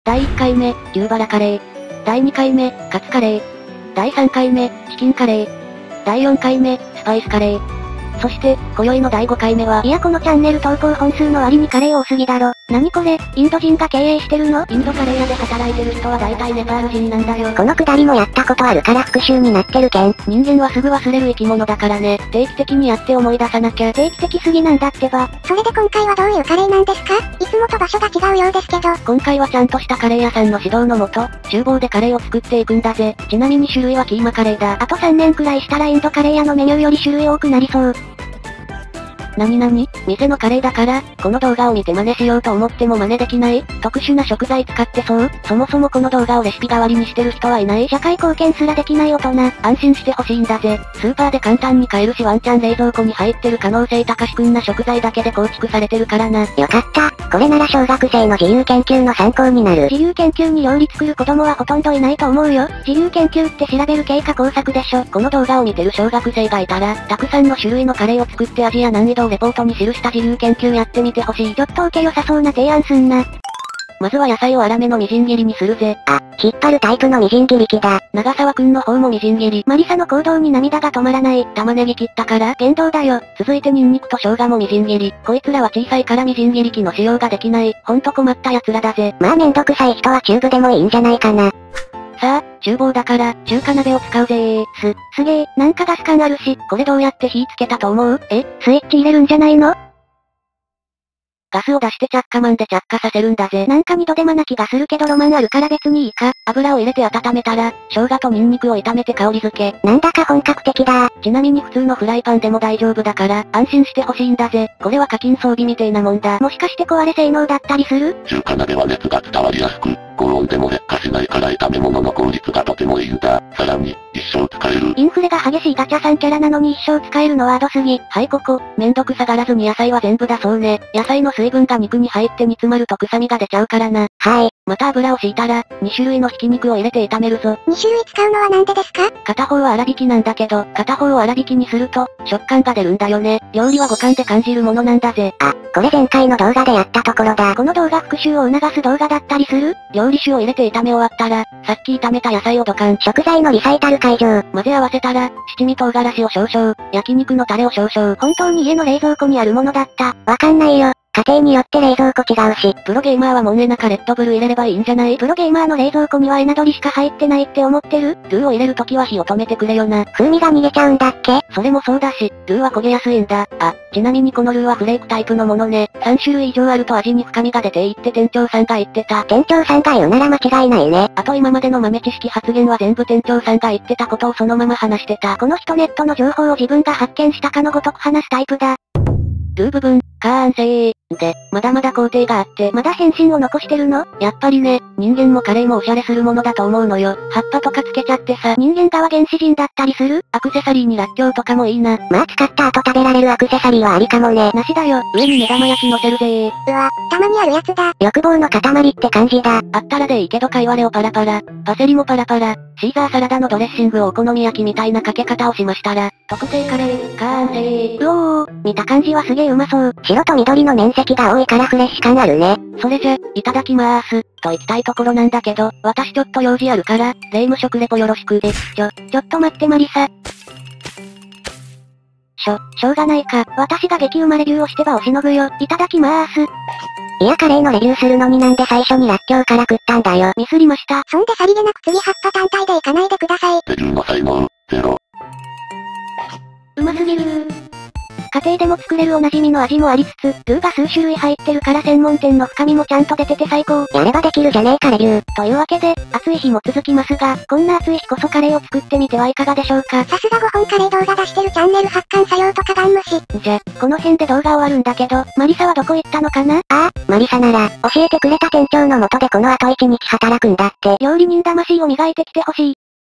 【キーマカレー】魔理沙ちゃんはキーマカレーを作るようです【料理】【ゆっくり実況】
これは一日店長企画をさせていただいたときの料理です！(概要欄に詳細なレシピあり)　ついでに店長さんの動画もあります

【レシピ】
4人〜5人前分
【材料】
・豚挽肉　200g
・粗挽き（豚・牛）200g
・玉ねぎ（2個）
・ニンジン（1本）
・玉子
・レタス
・カイワレ
・らっきょ
・バセリ粉
・シーザードレッシング
【調味料】
・市販カレーのルー　３種以上あると深みがでる。
　（フレークタイプを選ぶ）
・七味（少々）
・生姜（一欠片）
・ニンニク（一欠片）
・焼肉のタレ（30ml）
・料理酒（50ml）